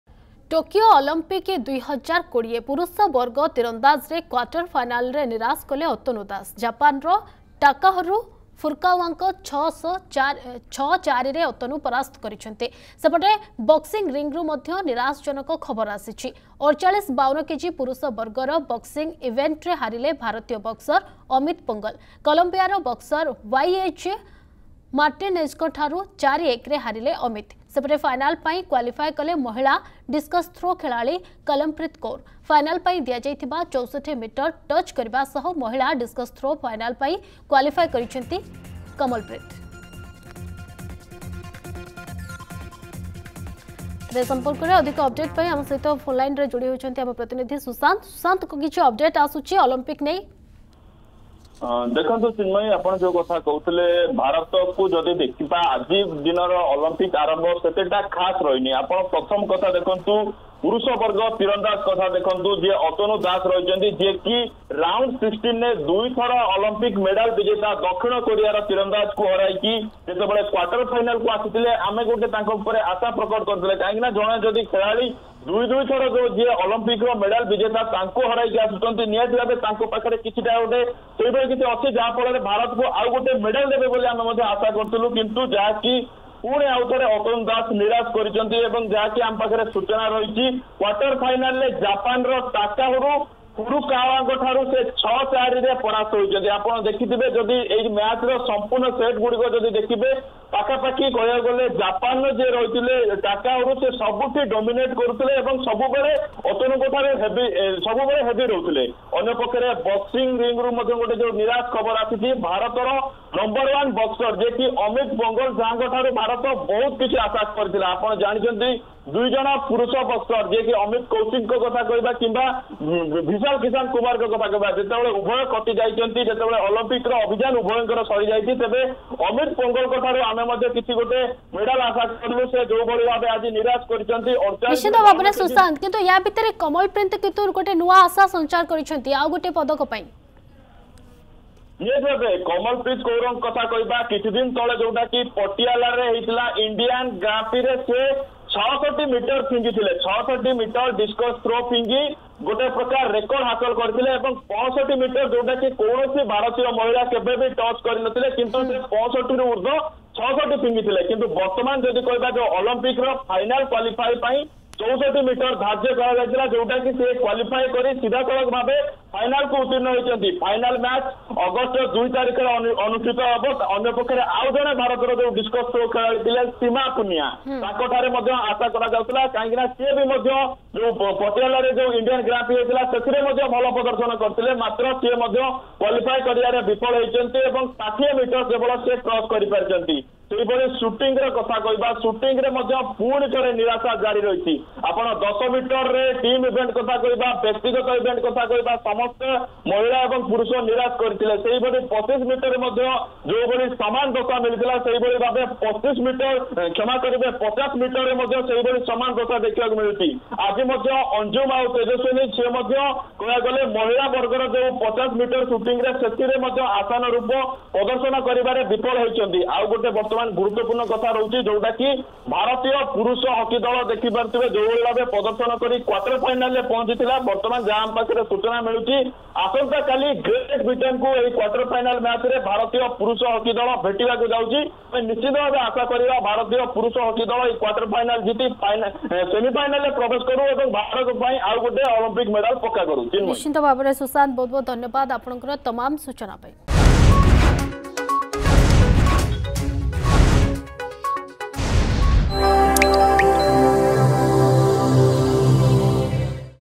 ଆର୍ଚେରୀ ବ୍ୟକ୍ତିଗତ ଇଭେଣ୍ଟ ପ୍ରି-କ୍ବାର୍ଟରରେ ହାରିଲେ ଅତନୁ ଦାସ
ଟୋକିଓ ଅଲମ୍ପିକ2020 । ପୁରୁଷ ବର୍ଗ ତୀରନ୍ଦାଜରେ କ୍ଵାଟର ଫାଇନାଲରେ ନିରାଶ କଲେ ଅତନୁ ଦାସ । ଜାପାନର ଟାକାହରୁ ଫୁରୁକାୱାଙ୍କ 6-4 ରେ ଅତନୁ ପରାସ୍ତ ହୋଇଛନ୍ତି। ସେପଟେ ବକ୍ସିଙ୍ଗ ରିଙ୍ଗରୁ ମଧ୍ୟ ନିରାଶଜନକ ଖବର ଆସିଛି।